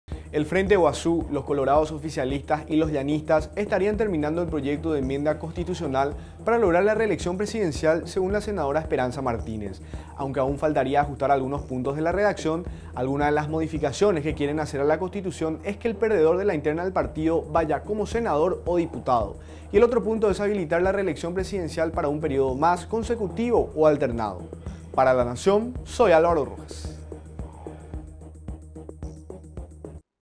Proyecto de enmienda estaría casi terminado
El Frente Guasu, los colorados oficialistas y los llanistas estarían terminando el proyecto de enmienda constitucional para lograr la reelección presidencial